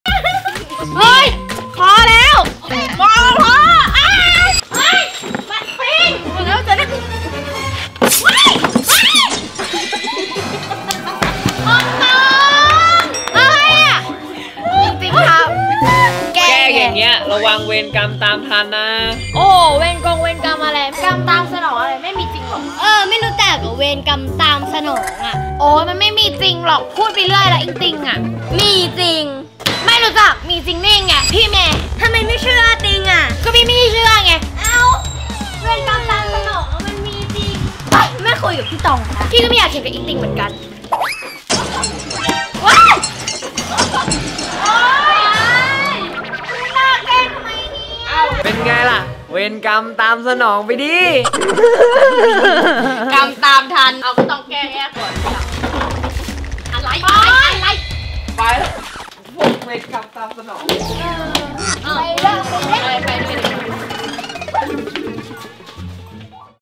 อองตองแกล้งเพื่อน แต่ตกเก้าอี้เอง I ละครสั้น ❤ ตอง ติง โชว์ ❤
Line : yoowipada

❤ ติดตามเราได้ที่นี่นะคะ
FACEBOOK  : ตอง ติง โชว์
FAN PAGE :  ตอง ติง โขว์